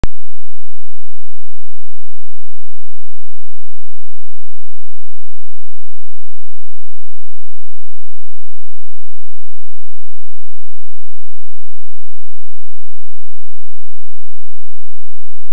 跨越雙黃線超車,速度最少80